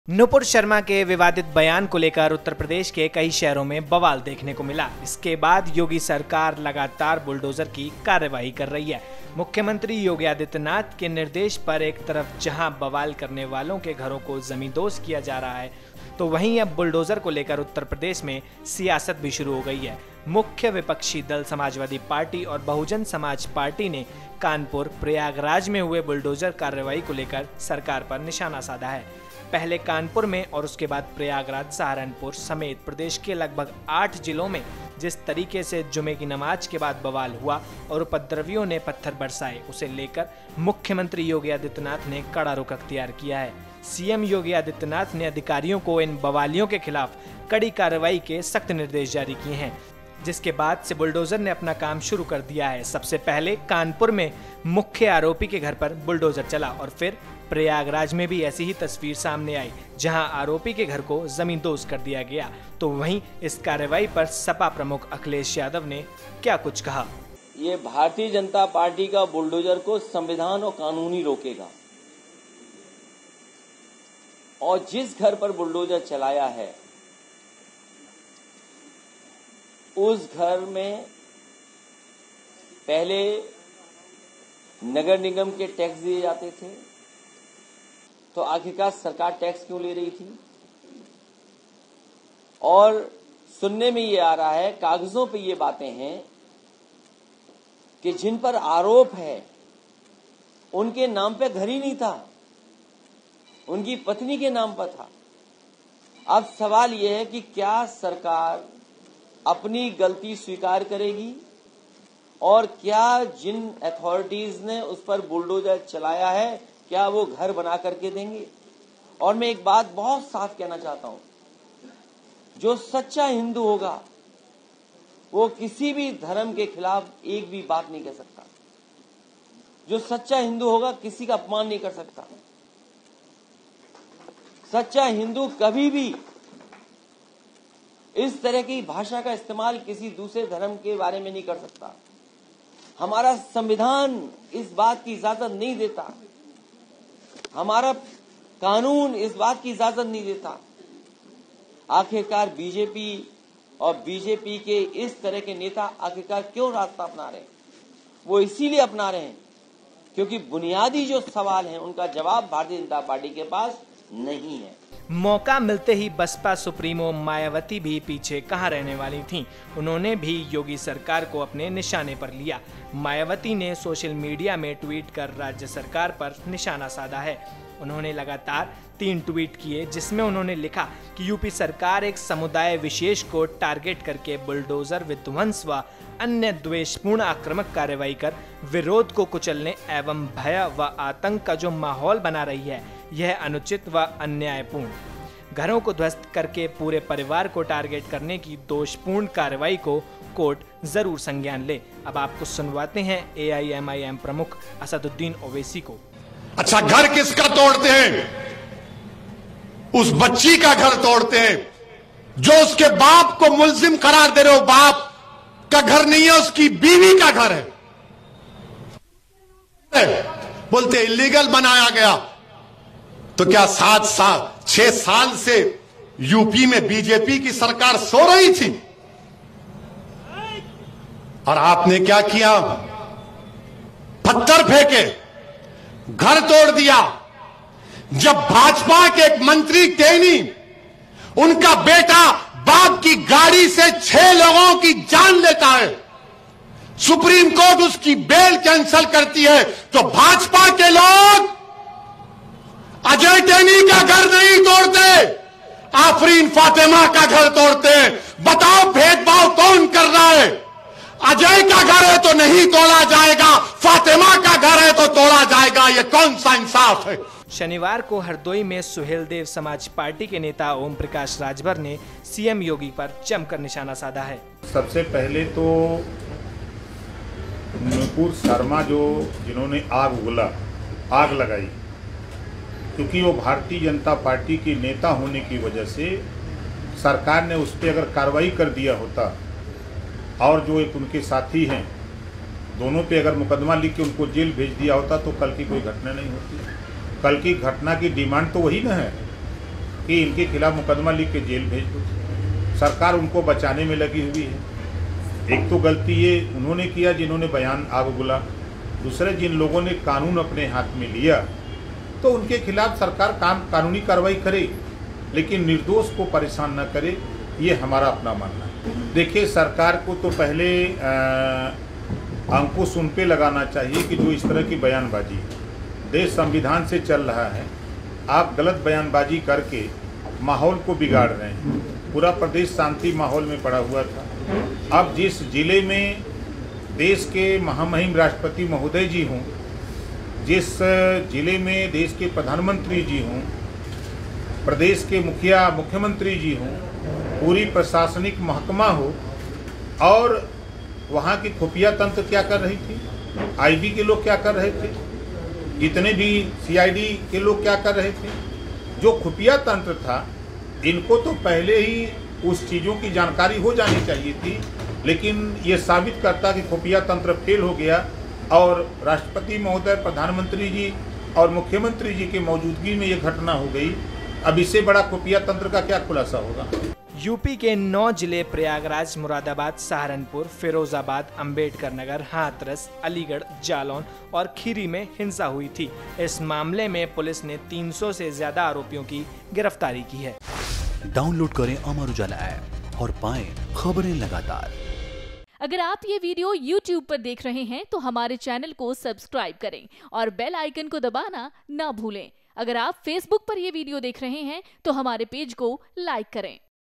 Yogi Adityanath के Bulldozer पर क्या बोले ये बड़े नेता| Akhilesh Yadav | Mayawati | UP News in hindi
उत्तर प्रदेश के कई शहरों में बवाल के बाद सरकार लगातार बुलडोजर की कार्रवाई कर रही है. मुख्यमंत्री योगी आदित्यनाथ (Yogi Adityanath) के निर्देश पर एक तरफ जहां बवाल करने वालों के घरों को जमींदोज किया जा रहा है, तो वहीं अब बुलडोजर को लेकर उत्तर प्रदेश में सियासत भी शुरू हो गई है. मुख्य विपक्षी दल समाजवादी पार्टी (SP) और बहुजन समाज पार्टी (BSP) ने कानपुर, प्रयागराज में हुए बुलडोजर कार्रवाई को लेकर सरकार पर निशाना साधा है.

After the ruckus in many cities of Uttar Pradesh, the government is continuously taking action of bulldozers. On one hand, on the instructions of Chief Minister Yogi Adityanath, where the houses of the rioters are being demolished, on other hand politics has also started in Uttar Pradesh regarding bulldozers. The main opposition parties Samajwadi Party (SP) and Bahujan Samaj Party (BSP) have targeted the government over the bulldozer action in Kanpur, Prayagraj.